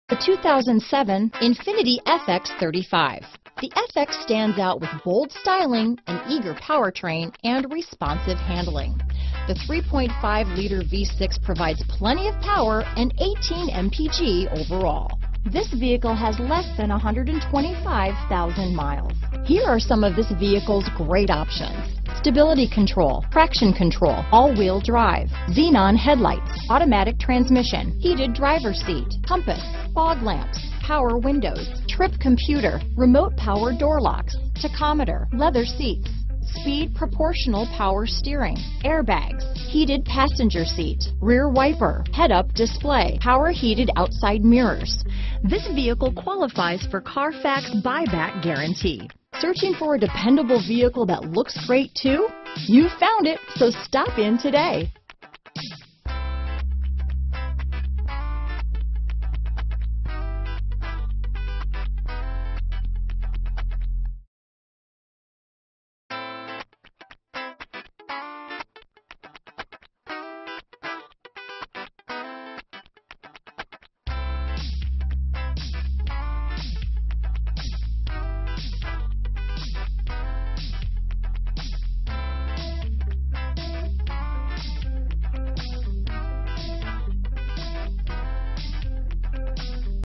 2007 Infiniti FX35 Fall River, Dartmouth, New Bedford, Wareham, MA, Tiverton, RI W716
Metallic  2007 Infiniti FX35 available in Fall River, Massachusetts at Prestige Auto Mart 1. Servicing the Dartmouth, New Bedford, Wareham, MA, Tiverton, RI area.

Includes a CARFAX buyback guarantee... All Wheel Drive!!!AWD! Safety equipment includes: ABS, Xenon headlights, Traction control, Curtain airbags, Passenger Airbag...Other features include: Leather seats, Power locks, Power windows, Heated seats, Auto...